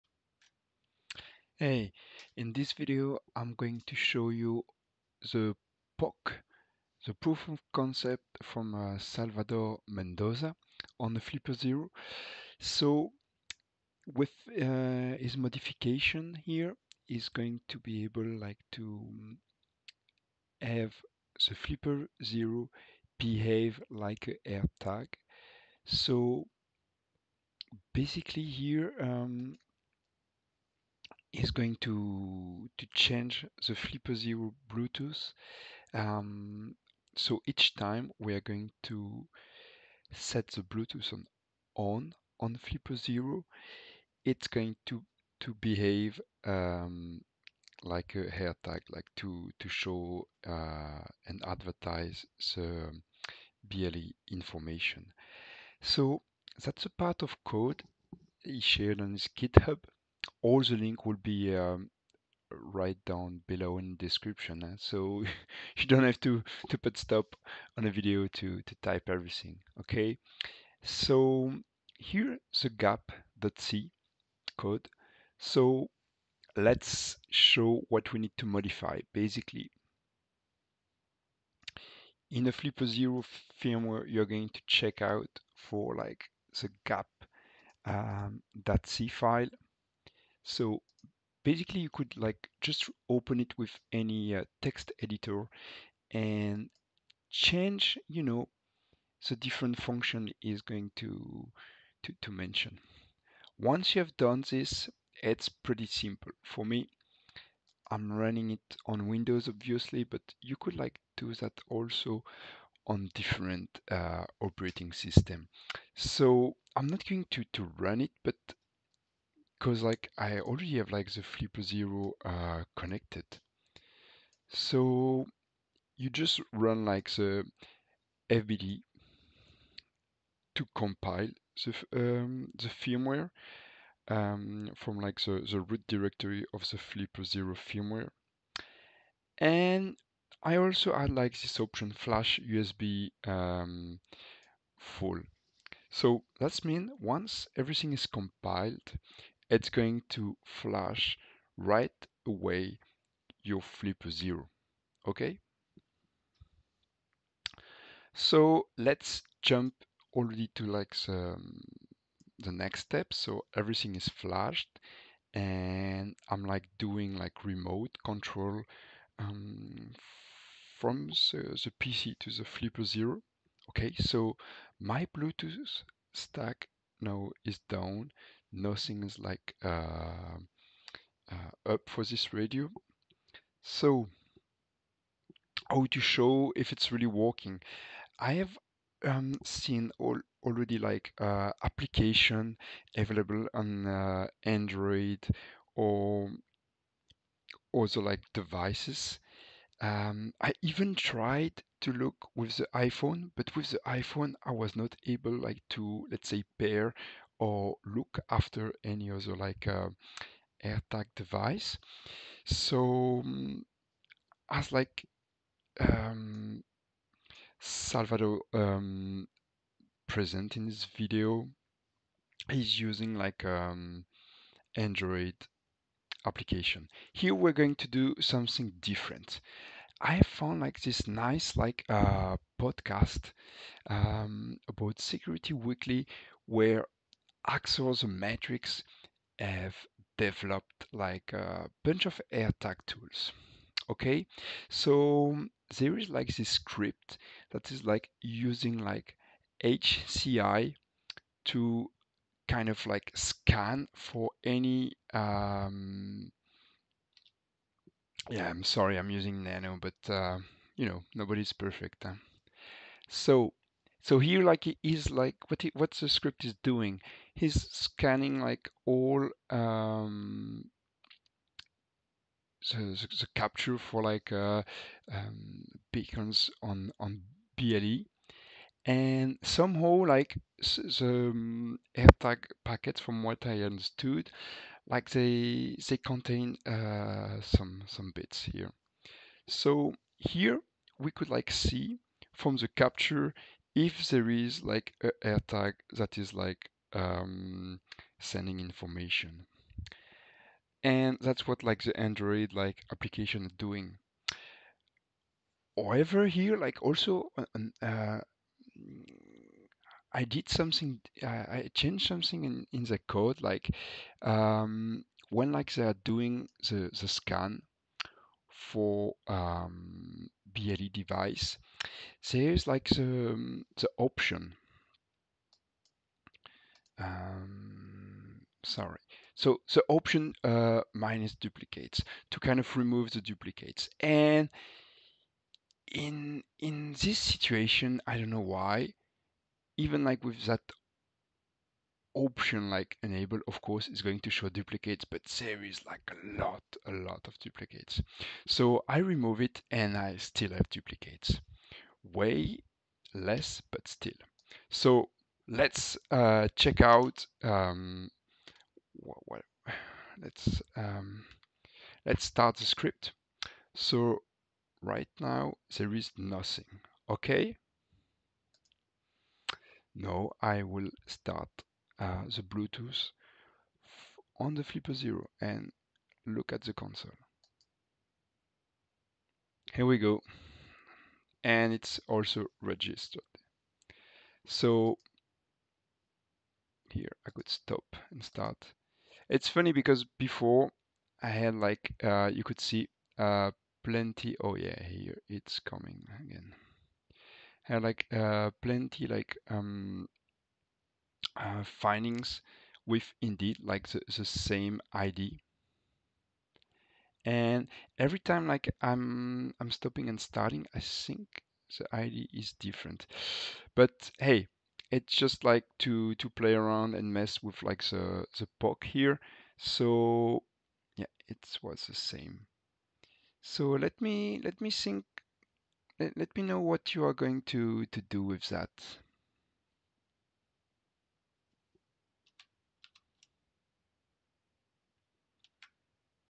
Flipper Zero : Mimic Apple AirTag advertising behavior Poc Walkthrough
Thanks to BadSmolivOil to bring visibility about this topic on the Discord

2. Modified functions from gap.c

3. Flipper ressources

4. Ressources from @SecurityWeekly
Apple Airtag Detection & Simulation - PSW #749

4. Useful commands

My Linux was a raspberrypi 4. Somehow I had to force to stop and start the interface for the ble.
sudo hciconfig hci0 down
sudo hciconfig hci0 up

Hcidump was also missing
sudo apt-get install bluez-hcidump